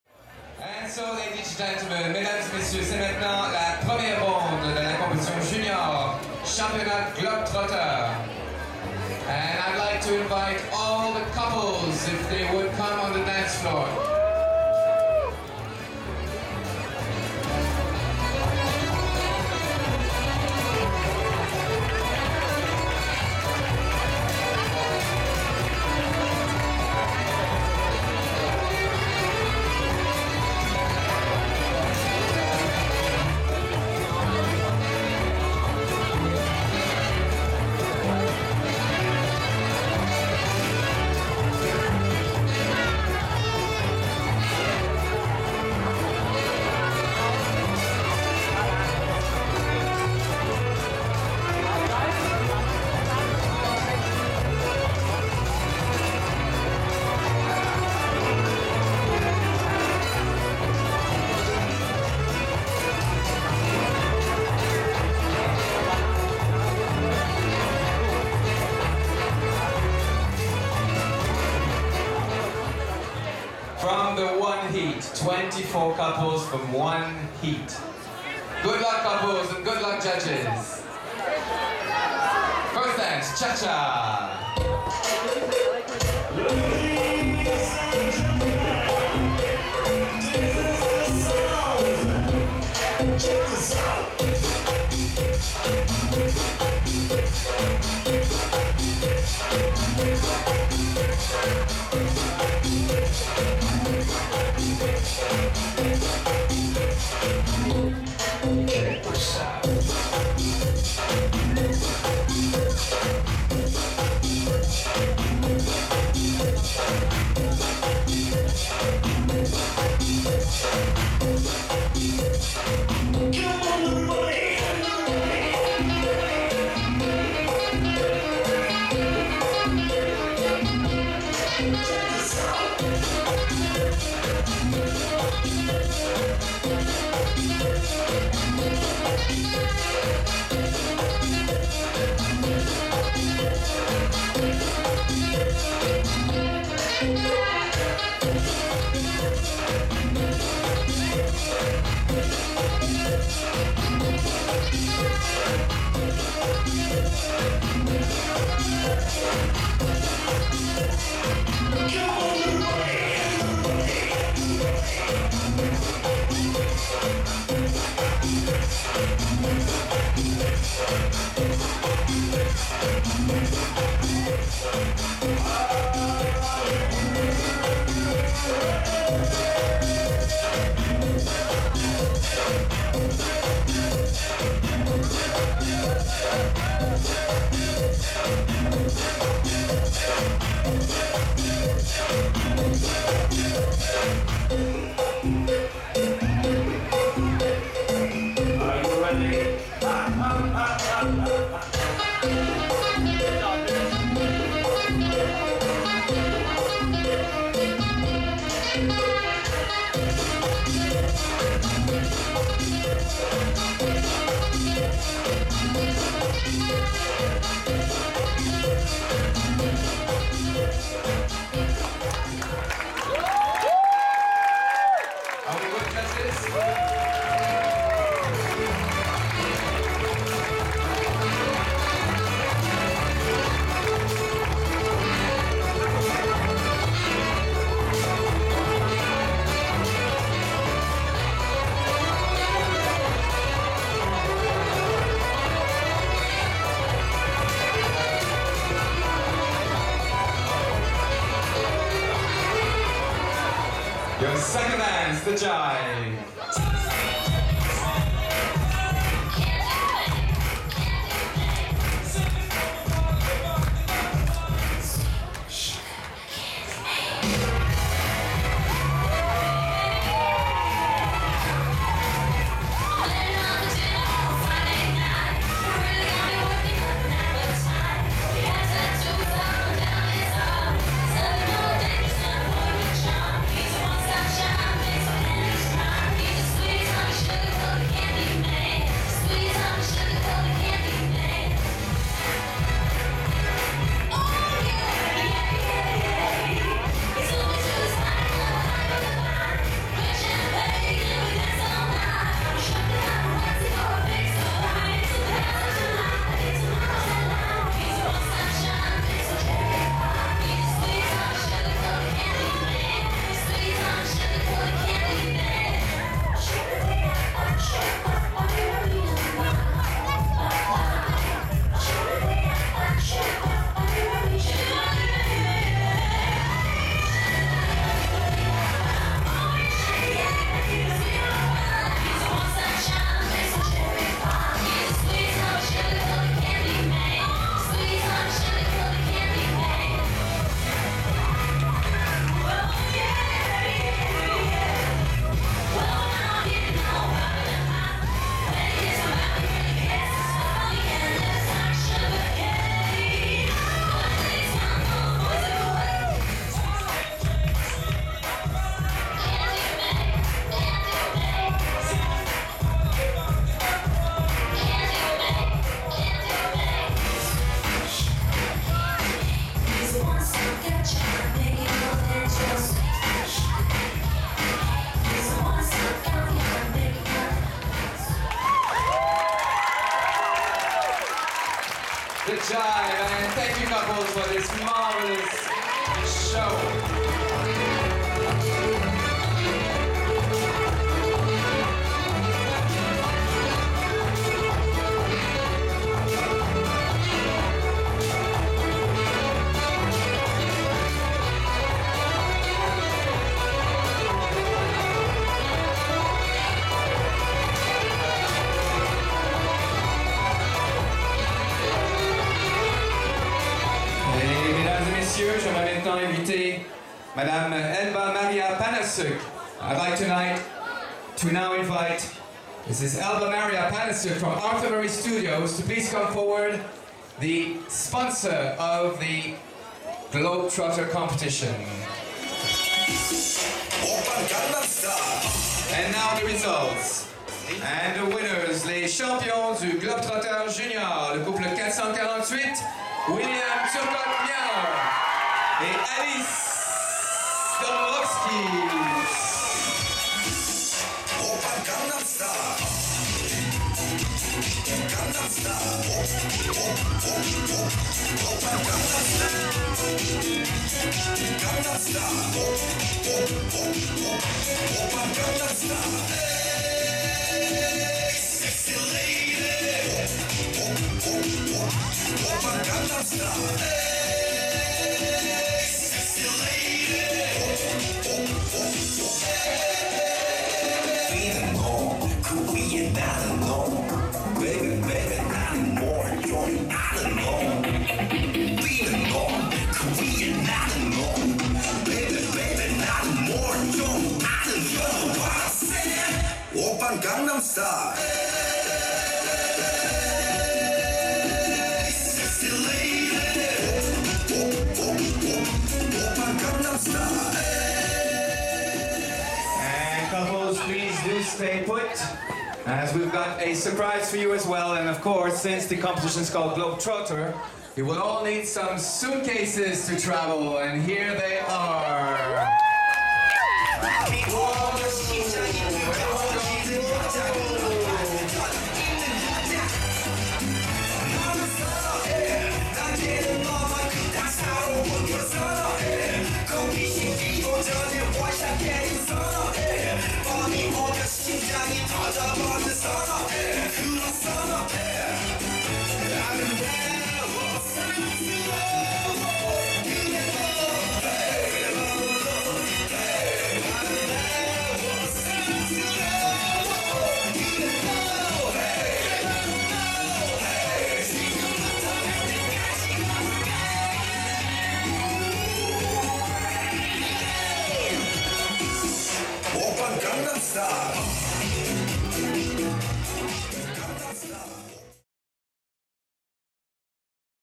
Globe Trotter à la Classique du Québec 2013 (full & award)
Spectacular 48 couples on 1 floor in the same time